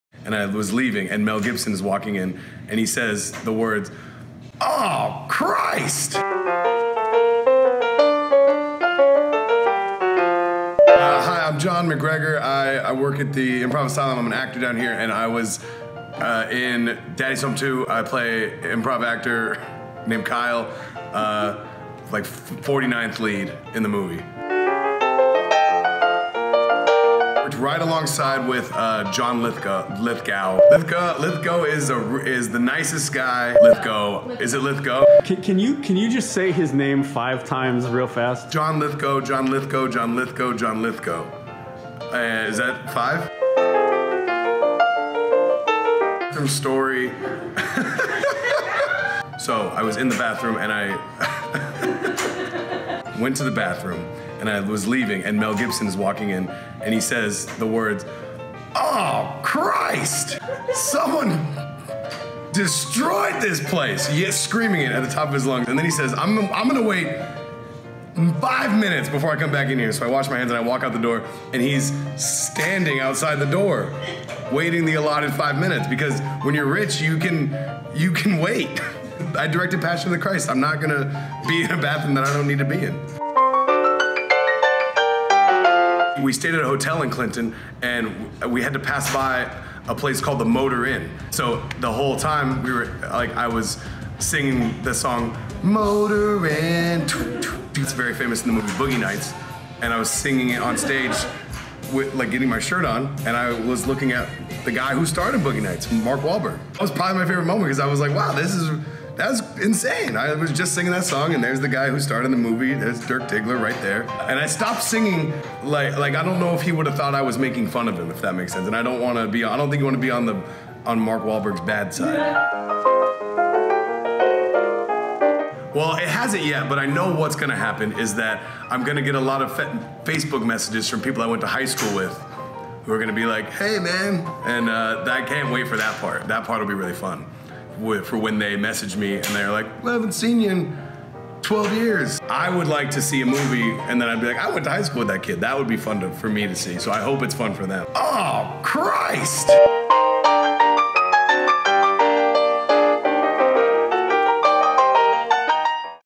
Tales From The Set: Daddy's Home 2 - John MacGregor
Improv Asylum Main Stage Actor John MacGregor shares stories from his time on the set of the Will Ferrell Mark Wahlberg film Daddy's Home 2, one of which includes Mel Gibson and a "destroyed" bathroom.